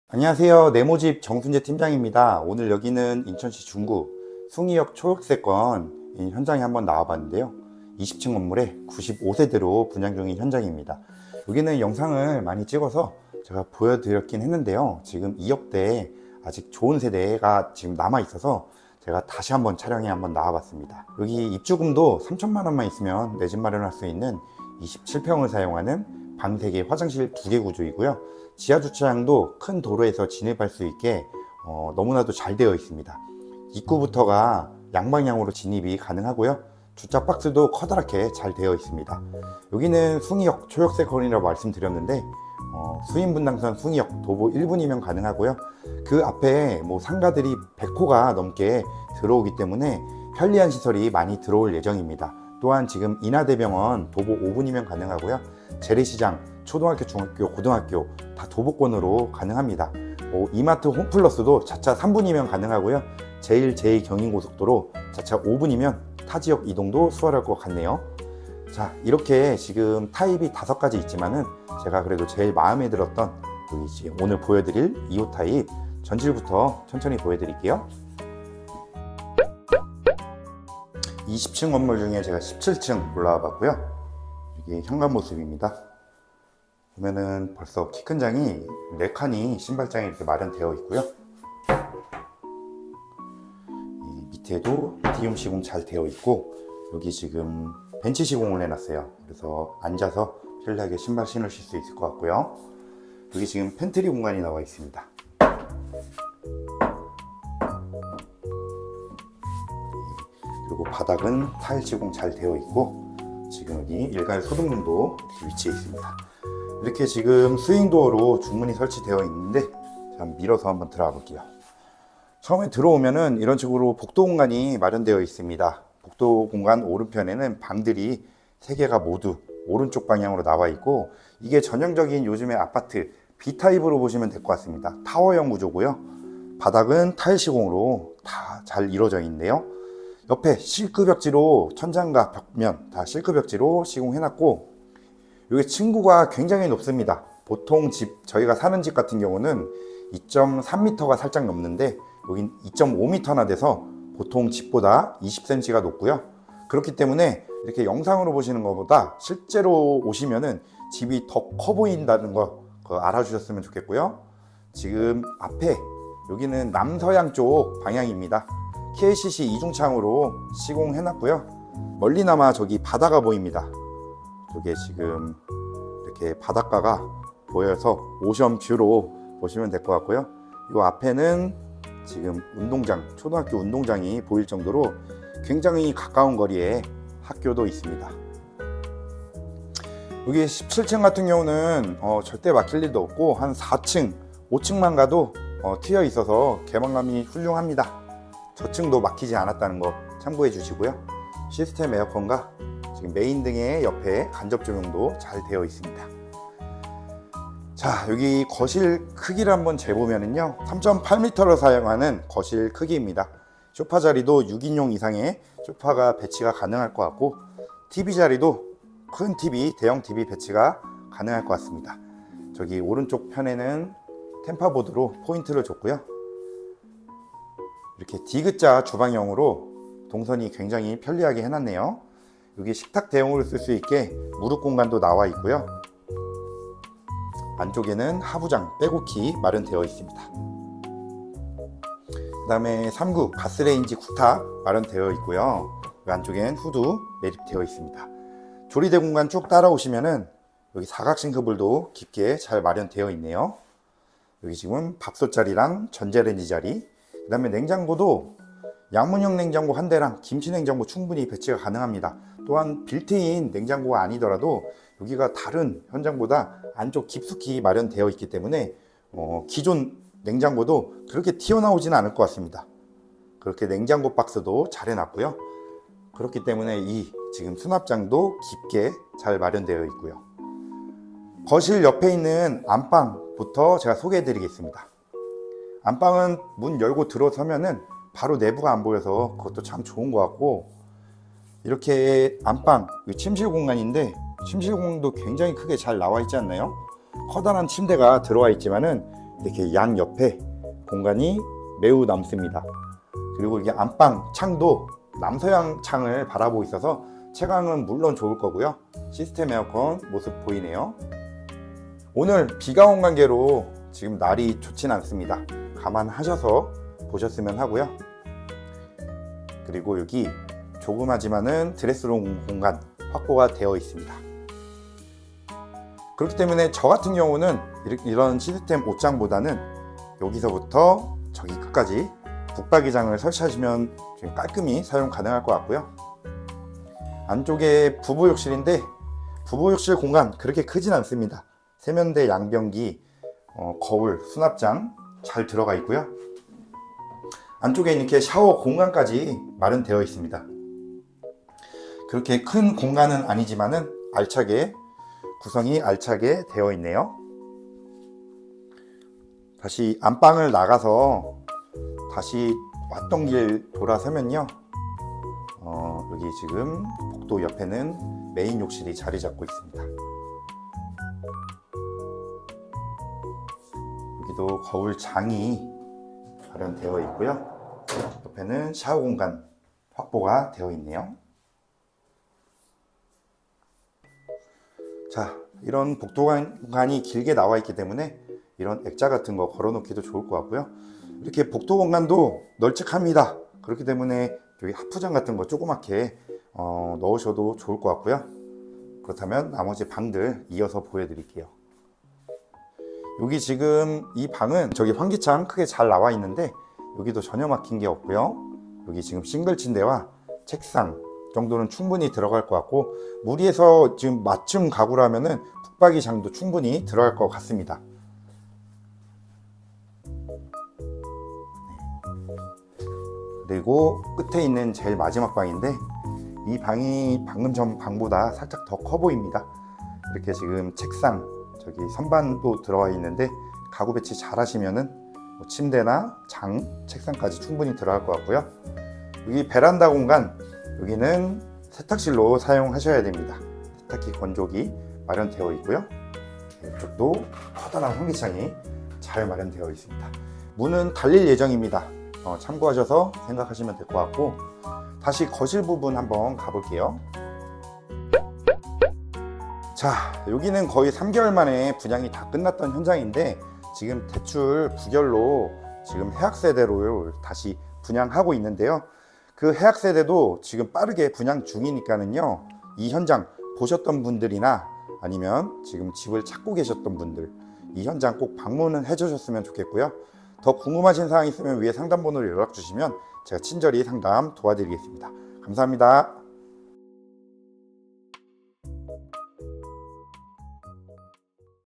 인천에서 분양속도 가장 빠른집!! 39평형 2억초중반대?! 마지막 최저가격😲 곧 준공 예정! 이번에 놓치면 후회하세요ㅠㅠ (매물번호 375)[인천신축아파트][인천미분양아파트]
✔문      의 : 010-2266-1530

✔위      치 : 인천 중구 신흥동

✔교      통 :
수인분당선 숭의역 도보 1분
제1,2경인 고속도로 자차 5분

✔분양가격 : 2억대
(🙏은행 대출 감정으로 기존 계약자분들이나 입주 세대에 영향을 미칠 수 있어 온라인상 정확한 금액 노출은 어려우니 유선문의 부탁드립니다!🙏)

✔필요 입주금 : 입주금 2,000만원~

✔구조 : 방3개 화2개 베란다1개

✔사이즈 : 27평

✔옵션 :
시스템에어컨 2대
전열교환기
펜트리공간

✔주변환경 :
인천신광초 도보 1분
이마트 도보 6분
인하대병원 도보 6분
재래시장 도보 3분

✔특징 : 신규오픈, 저렴한 분양가, 안방드레스룸1개